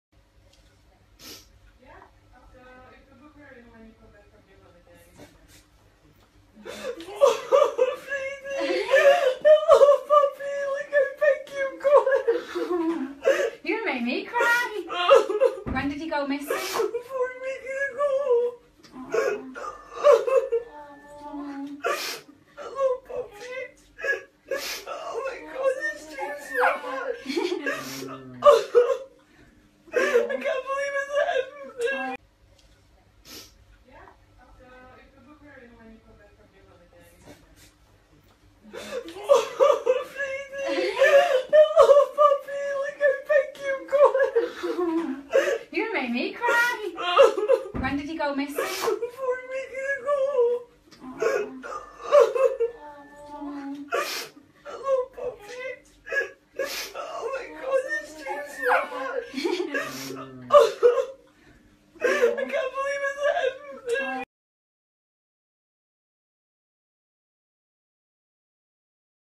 Emotional moment Aime Sands is reunited with her dog - Daily Mail
Brady, a spaniel, poodle, cairn terrier cross, was only 14 weeks old when he was taken from Aime Sands' back garden in Dunbar, East Lothian a month ago. He was found after he jumped into the back of a woman's car in Ashton-under-Lyne, Manchester on Thursday. The woman, Sue Travis, handed him to a nearby veterinary practice. Gourley Veterinary Surgeons checked the microchip and contacted Ms Sands, who initially thought it was a hoax. Ms Sands' mum videoed the touching moment her daughter saw her puppy - a spaniel, poodle, cairn terrier cross - for the first time since he went missing.